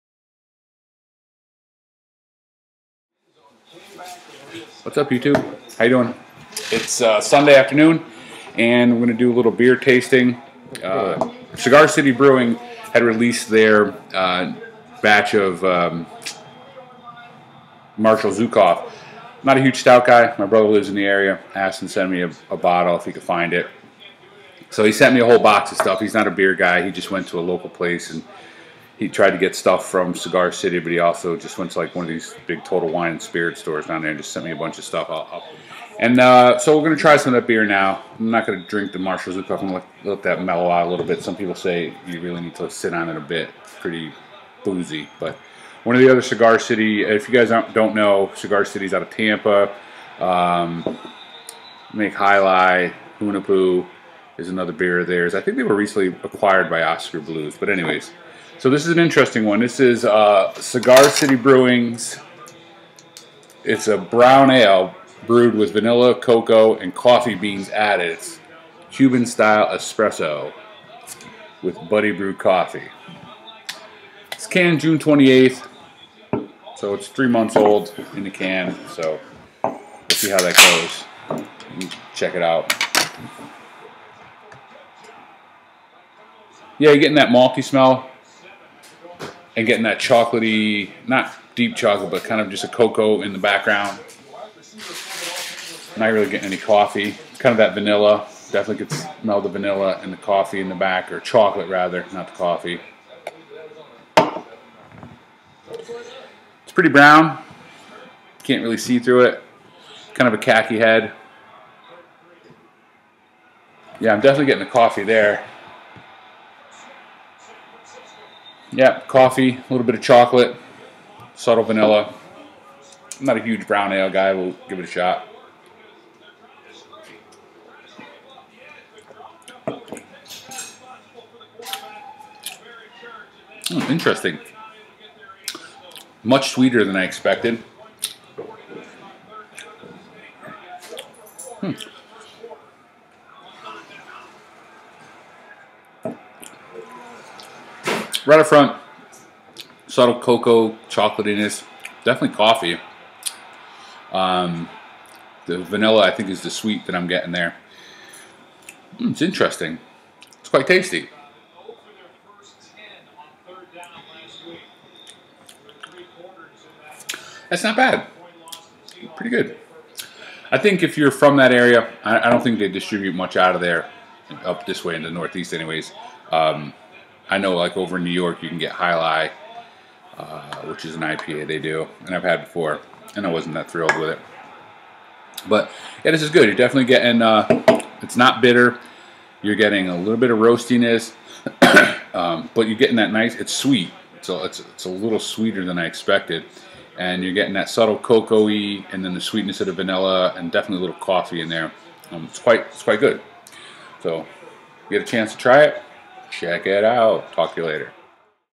Cigar City Brewing's "Cubano-style Espresso
I try this offering from Tampa's Cigar City brewing.